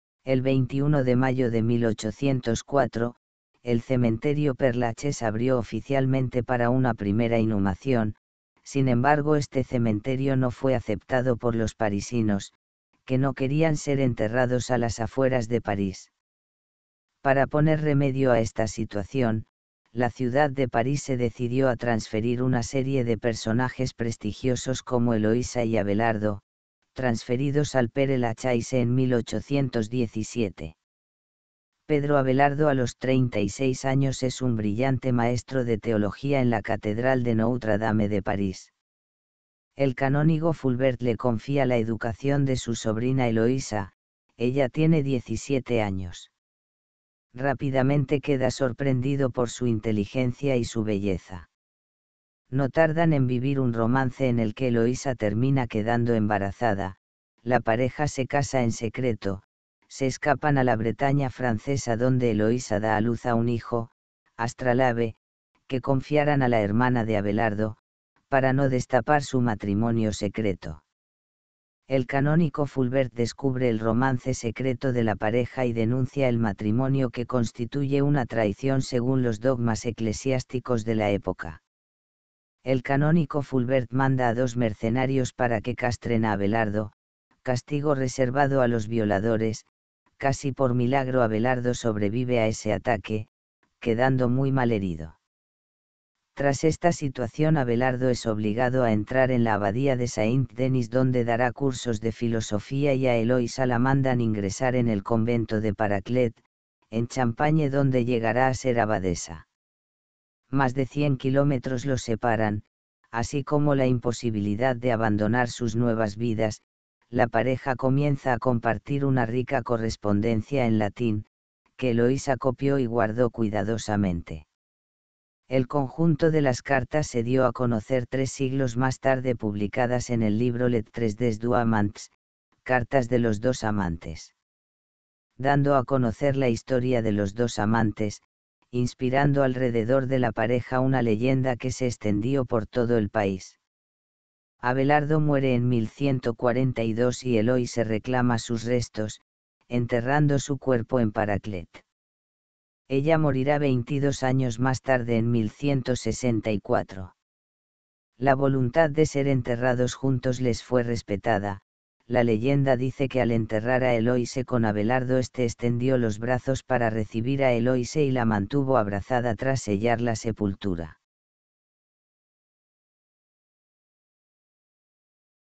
ELOISA Y ABELARDO una história de París
breve história de Heloïsa y Abelardo, estos dos amantes franceses del siglo XII vivieron un romance similar al de Romeo y Julieta, sirviendo de inspiracón a la literatura francesa de los siguientes siglos.